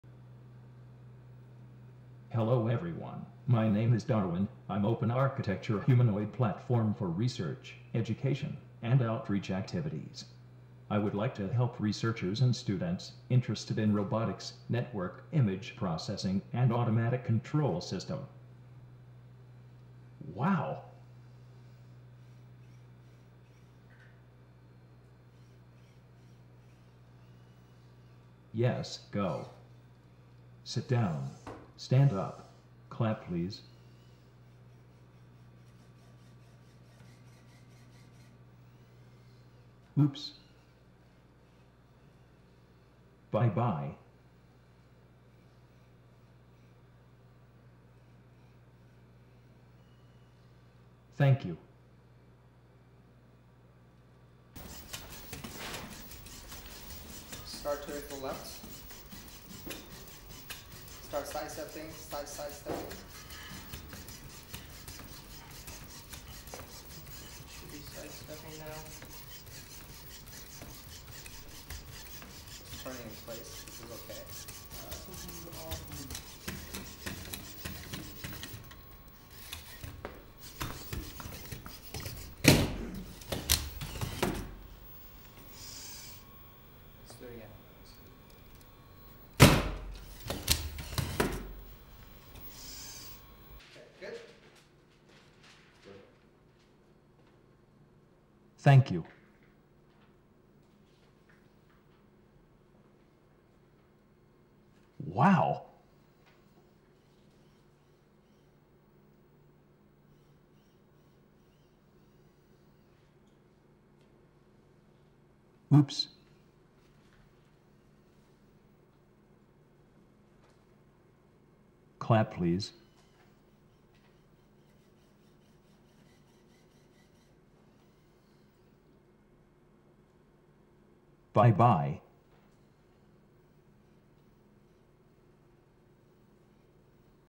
Virginia Tech: Introducing DARwIn-OP
Introducing Virginia Tech's Robotics and Mechanisims Laboratory's latest DARwIn-OP - Dynamic Anthropomorpic Robot with Intelligence, Open Platform, which means you can fabricate the parts, choose your own electronics and build one of your own.  Or, if you prefer, you can just buy one.